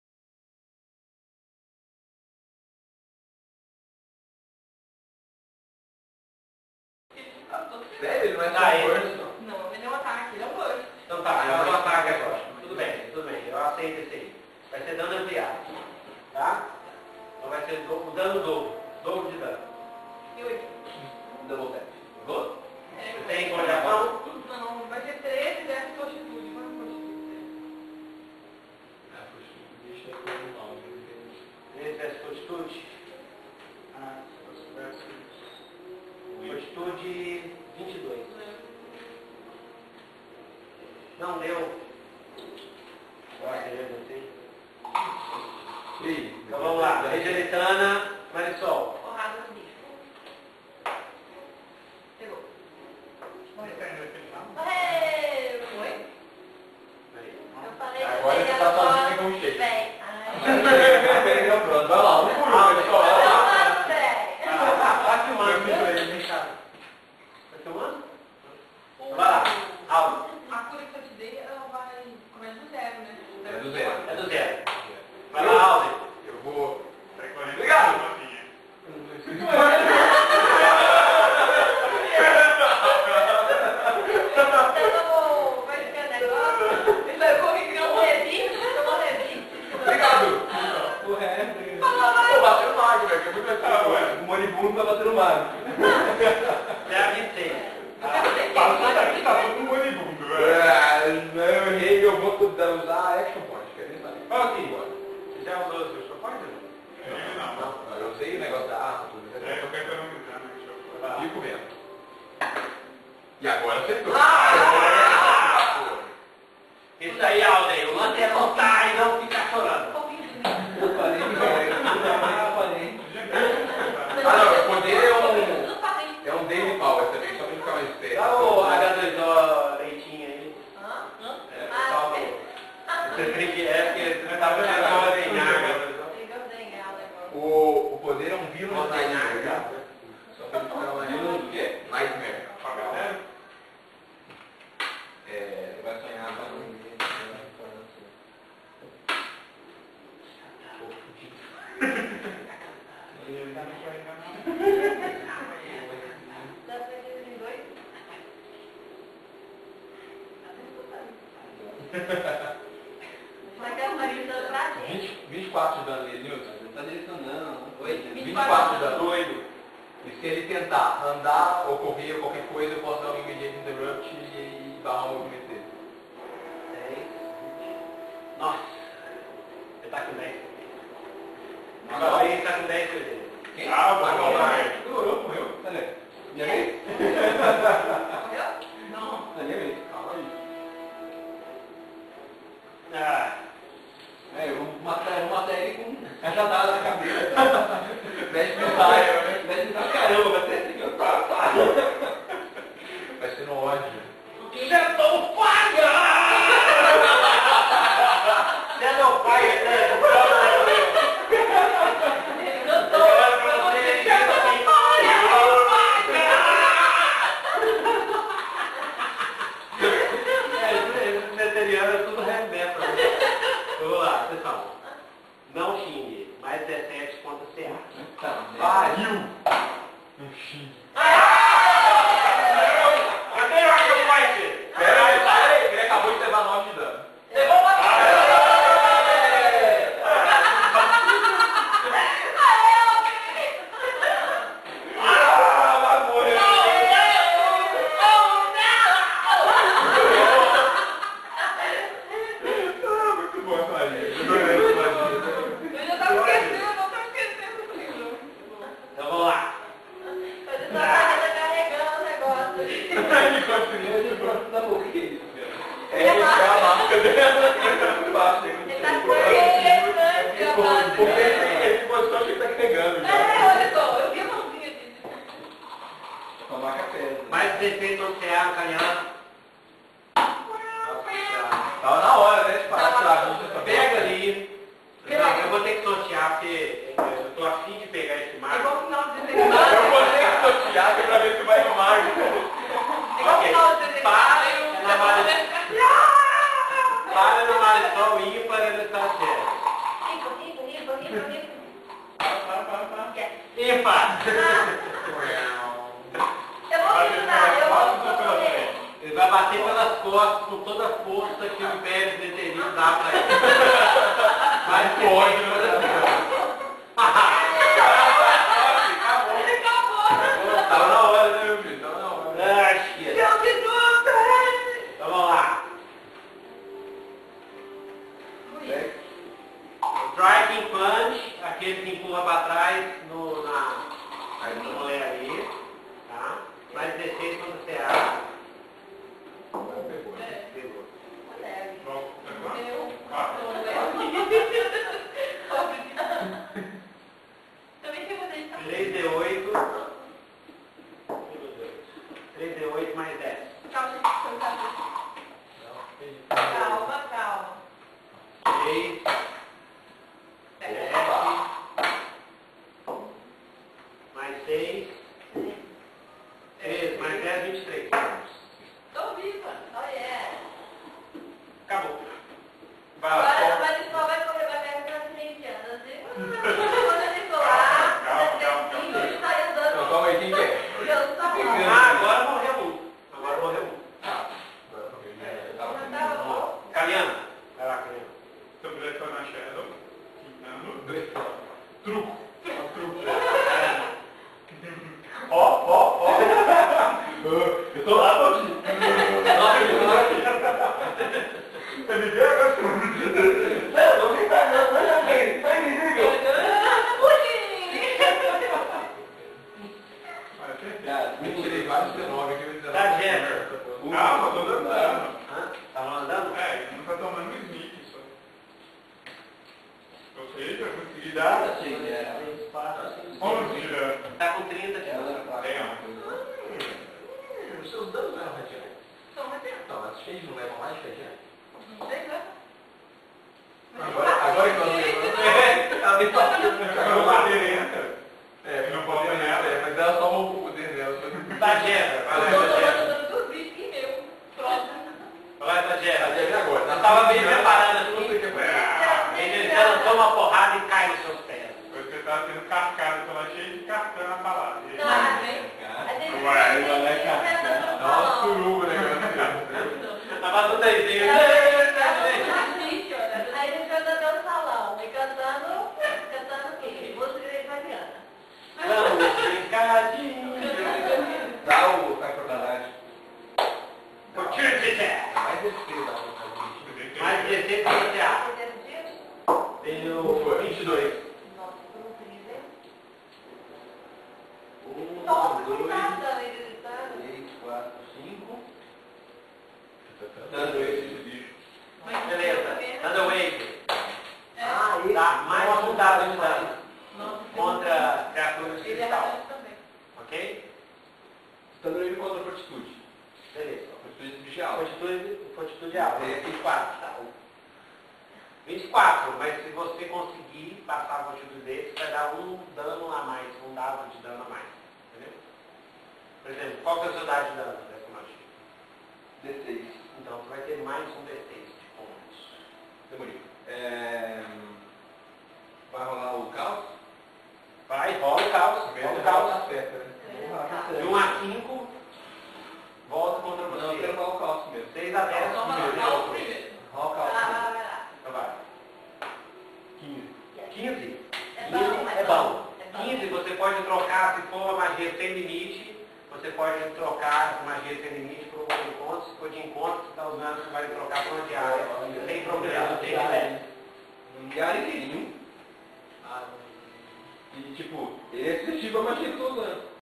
Combate na Torre do Cristal Negro! (Parte 1) - NitroDungeon RPG Blog
Vídeo de parte do 15º Episódio da nossa campanha "Império das Sombras" (Dungeons and Dragons 4ª Edição e Forgotten Realms).

O jogo foi bem legal, com combates desafiadores, confira o diário de campanha no NitroDungeon RPG Blog

Um abraço do Tio Nitro! :)